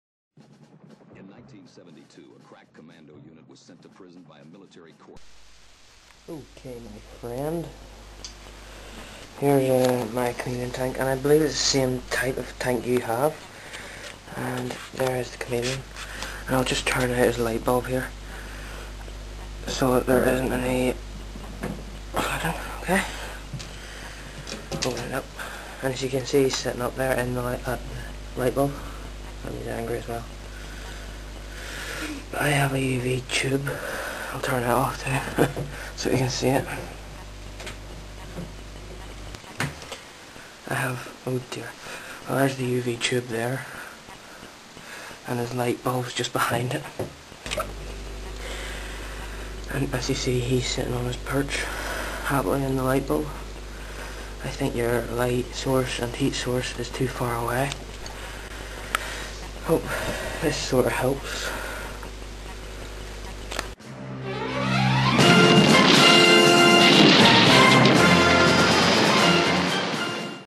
Dave
Just showing my lighting for Daves tank !!!